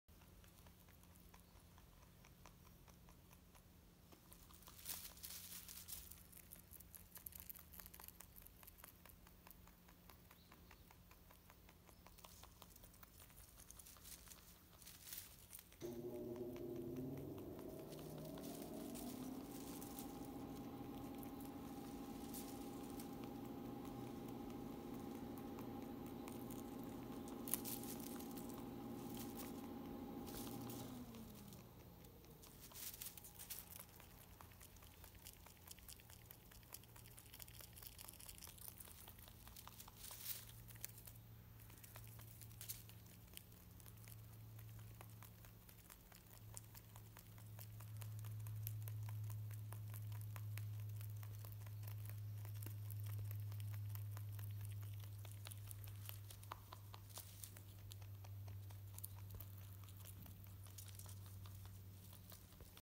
Rabbit eating oat hay.
We are making hay for rabbits in Japan.

Rabbits are delighted with the high quality hay made in Japan.